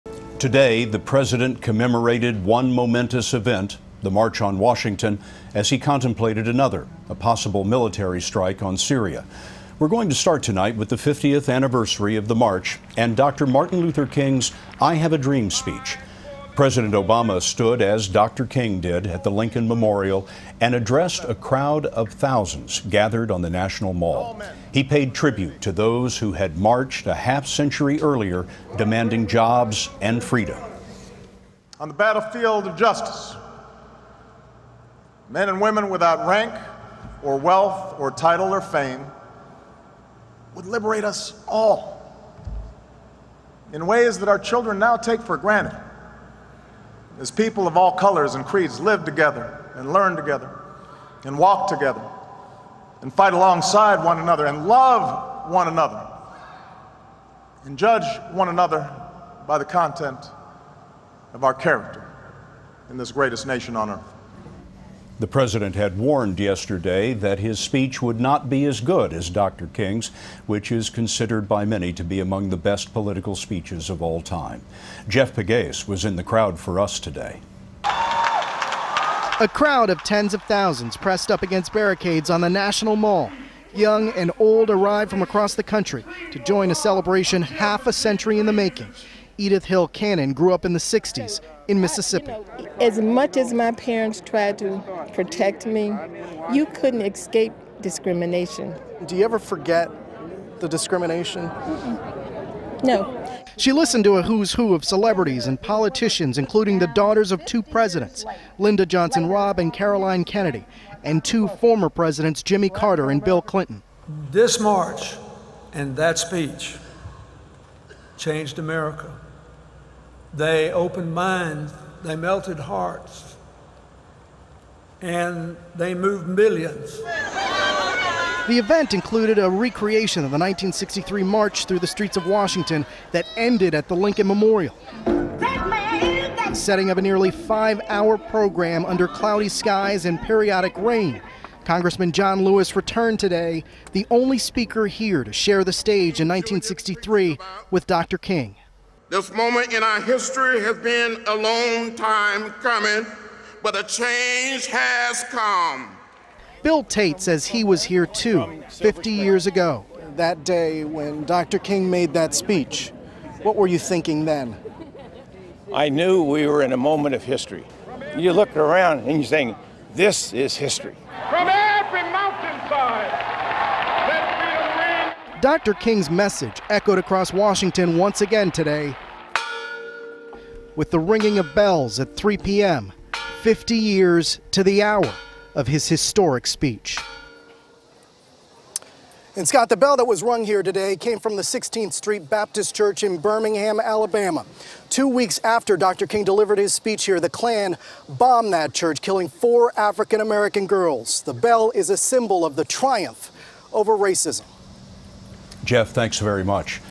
Nation honors Dr. Martin Luther King Jr.
Tens of thousands of people gathered in Washington, D.C., to celebrate the late civil rights leader and the 50th anniversary of the March on Washington. Speakers including President Obama and former President Clinton reflected on how the march and King's iconic speech changed history forever. Jeff Pegues reports.